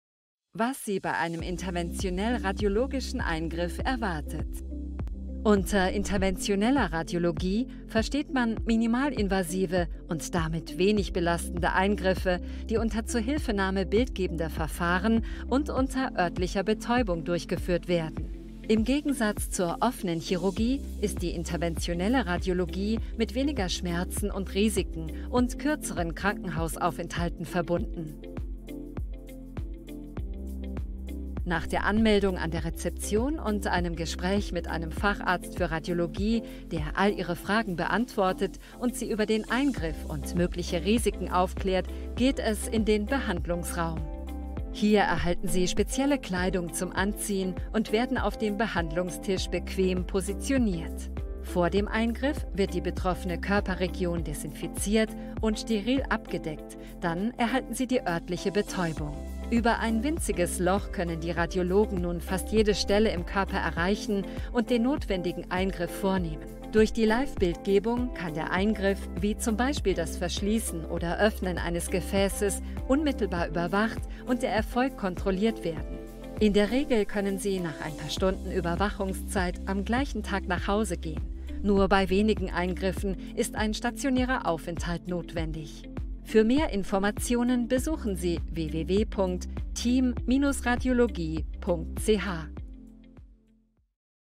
Team Radiologie Plus Erklärvideo INTERVENTIONELL-RADIOLOGISCHER EINGRIFF
Was Sie bei einer INTERVENTIONELL-RADIOLOGISCHEN Untersuchung erwartet